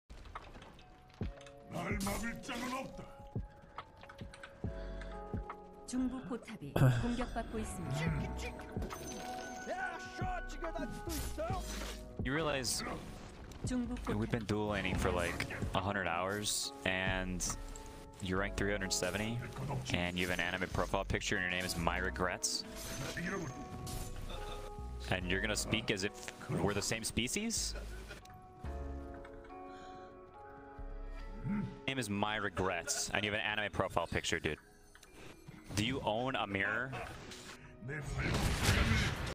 Quinn's response to pub player saying he lost mid to worst eu midlaner
Credits:
Dendi